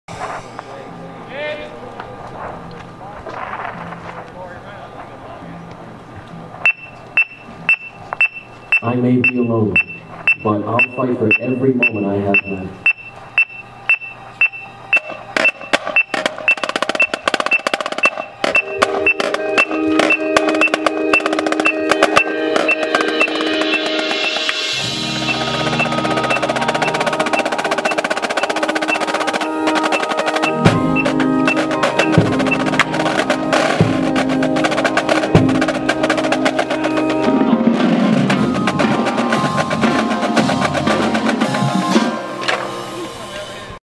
2009 MCM snare line rehearsing beginning of closer
Music City Mystique close up of the snare line doing the first part of the closer.